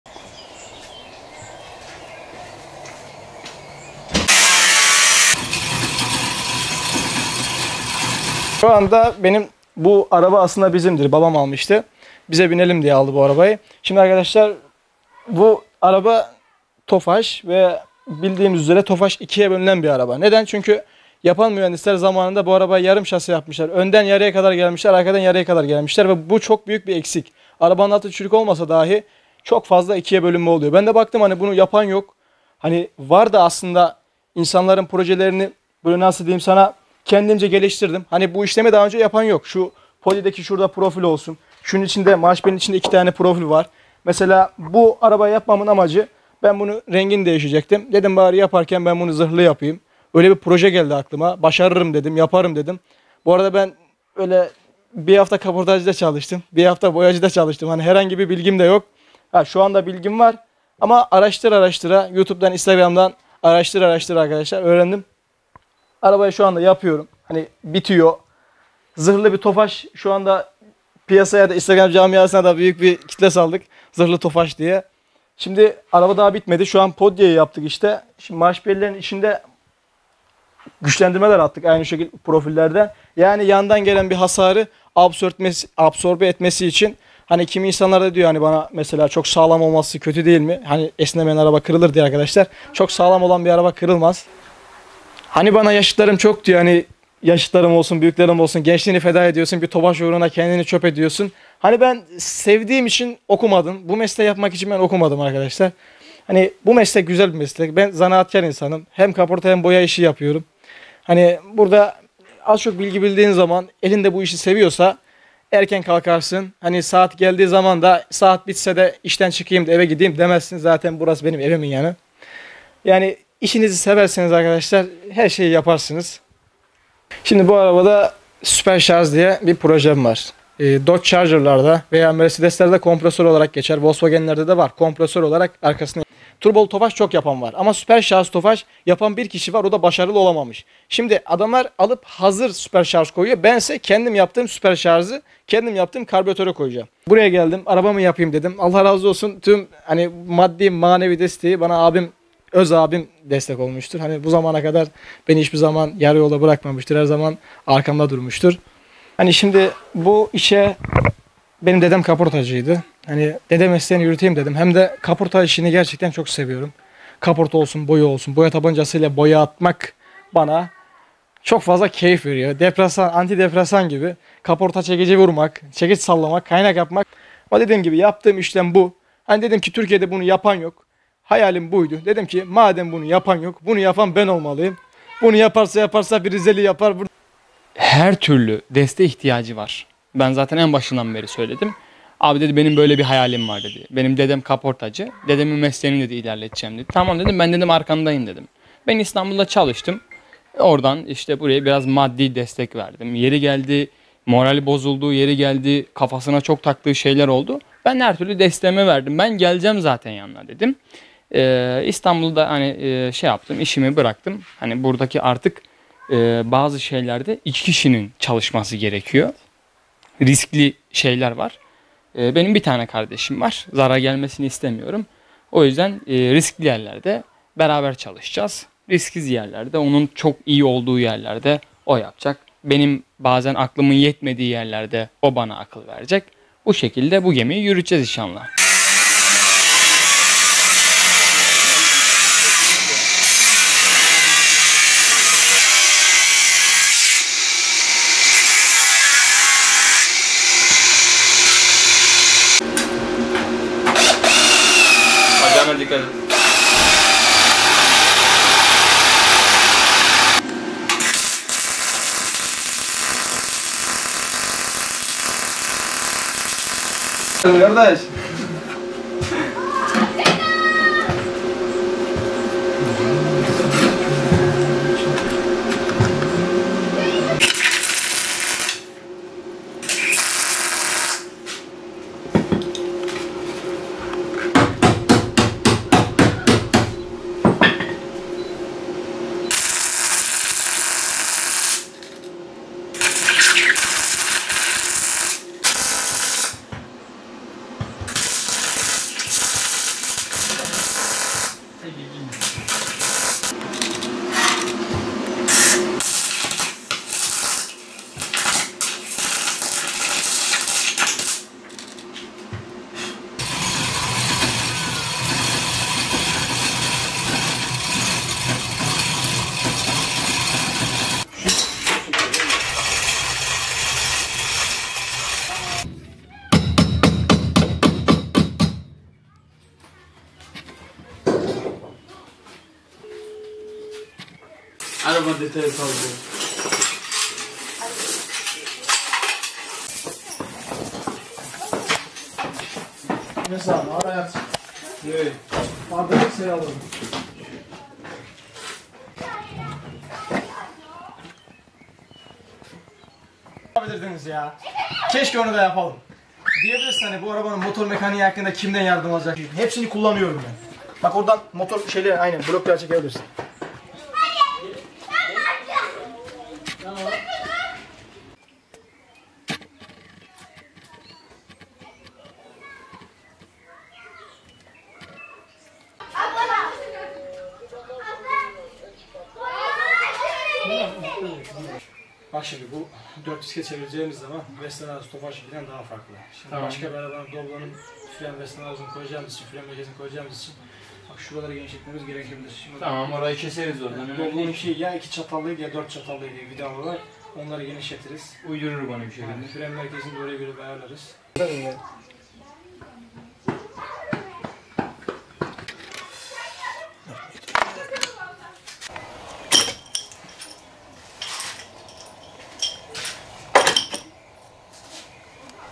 Zırhlı Tofaş! Çılgın Rizeli Dede Mesleğine Sahip Çıktı Türkiye'de Tek / AGRO TV HABER
Rize’de yaşayan 17 yaşındaki çocuk, eski model Tofaş araca kendi elleriyle hem zırh hem de motor yapıyor.

Rize’nin Kalkandere ilçesinde yaşayan 17 yaşındaki Salih Çakır, babasının yıllar önce aldığı Tofaş marka aracın ilk önce rengini değiştirmek istedi. Daha sonra arabada başka ne yapabileceği hakkında düşünen Çakır, Türkiye’de daha önce yapılmamış olan bir zırhlı Tofaş projesine karar verdi. Çalışmalara başlayan Çakır, öncesinde arabanın her yerini sökmeye başladı ve A’dan Z’ye bütün parçalarını sökerek zırh çalışmalarına başladı. Öncelikli olarak şase güçlendirmesi yapan Özcan sonrasında aracın çevresini demir profillerle kaplamaya başladı. Gün geçtikçe zırhla kaplamaya başladığı arabanın motorunu da kendi elleriyle yapmak isteyen Çakır, yeni model arabalarda bulunan supercharger motoru da kendi elleriyle yapmak için çalışmaya başladı. Hayatında önceden bir ustalık yapmadan bu çalışmaya başlayan Çakır, Türkiye’de ilk kez yapılan bu proje için kendisiyle gurur duyuyor.

“Bu işlemi daha önce Türkiye’de yapan yok”

Bu işlemi Türkiye’de daha önce yapanın olmadığını ifade eden Salih Çakır, “Babam bu arayı binelim diye almıştı. Bu araba Tofaş ve bildiğiniz üzere bu araç yarım şase olarak yapıldı. Bu araçta çok fazla ikiye bölünme meydana geliyor. Bu işlemi daha önce Türkiye’de yapan yok. Bu arabadaki profilleri kimse yapmadı. Ben önce bu arabanın rengini değişecektim. Sonrasında aklıma böyle bir proje geldi ve ben bunu başarırım dedim. Ben sadece 1 hafta kaportacıda ve boyacıda çalıştım. Fazla da bir bilgim yok. Şu anda bilgi sahibi oldum. Sosyal medyadan araştırarak bilgi sahibi oldum. Arabayı şu anda yapıyorum. Piyasaya da ’zırhlı Tofaş’ olarak nam saldık. Araba daha bitmedi. Bana çoğu kişi çok sağlam araba değil mi diye soruyor. Çok sağlam olan bir araba kırılmaz” şeklinde konuştu.

“Gençliğini feda ediyorsun diyorlar”

Türkiye’de bunun gibi bir aracın yapılmadığını görünce kendisinin bu projeyi yapmak istediğini belirten Çakır, “Bana yaşıtlarım ve çevremdekiler çok diyor. Gençliğini feda ediyorsun diyorlar. Bir Tofaş uğruna kendini çöp ediyorsun diyor. Ben sevdiğim için okumadım. Bu mesleği sevdiğim için okumadım. Ben zanaatkar bir insanım. Hem boya hem de koparta işlemi yaparım. Bu işi seviyorsan erken kalkarsın. Bu arabada benim supercharger motor denilen bir projem var. Turbolu Tofaş yapan çok var ama supercharger yapan bir kişi var oda başaramamış. Ben kendim yaptığım supercharger’ı kendim yaptığım karbüratöre koyacağım. Buraya geldiğimde arabayı buraya koydum. Bu zamana kadar bana öz abim destek olmuştur. Beni hiç yalnız bırakmadı. Benim dedem kaportacıydı. Dede mesleğini yürüteyim dedim. Boya tabancasıyla boya yapmak bana keyif veriyor. Antidepresan gibi geliyor. Benim yaptığım işlem bu. Türkiye’de bunu yapan yok. Madem bunu yapan yok, bunu yapan ben olmalıyım dedim. Bunu da yaparsa yaparsa Rizeli yapar” ifadelerini kullandı.

“Ben her türlü desteğimi verdim”

Ağabeyi olarak Salih’e maddi ve manevi her türlü desteği vermeye çalıştığını söyleyen Muhammet Çakır, “Her türlü desteğe ihtiyacı vardı. Bana ’abi böyle bir hayalim var’ dedi. ’Dedemin mesleğini ilerleteceğim’ dedi. Ben arkandayım dedim. Ben İstanbul’da çalıştım. Maddi destek verdim. Yeri geldi morali bozuldu. Ben her türlü desteğimi verdim. İstanbul’da işimi bıraktım. Burada bazı şeylerde artık iki kişinin çalışması gerekiyor. Riskli şeyler var. Benim bir tane kardeşim var. Zarar gelmesini istemiyorum. Bu şekilde bu gemiyi yürüteceğiz” dedi.